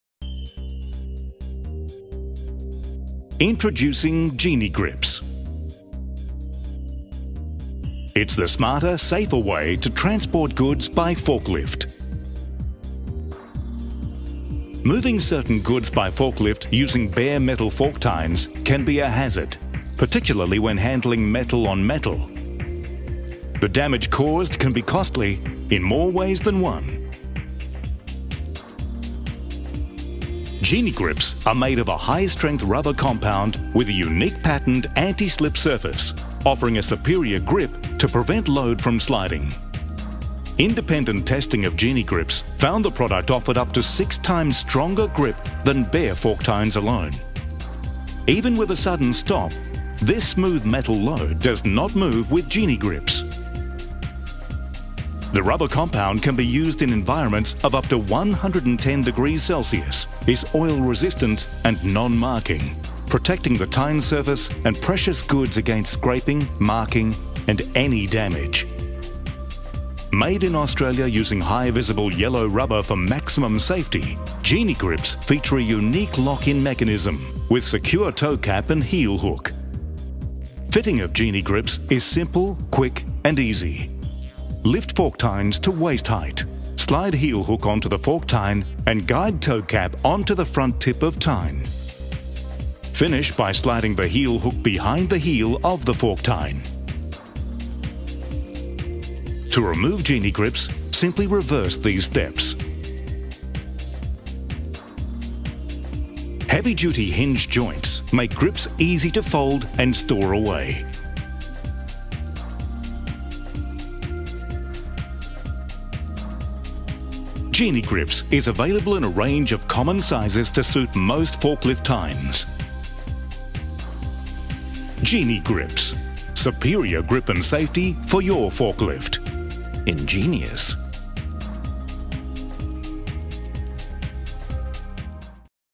GenieGrips Anti Slip Forklift Safety Mats
GenieGrips forklift safety products are high quality, durable, products for forklifts that provide safe, secure and stable movement of loads.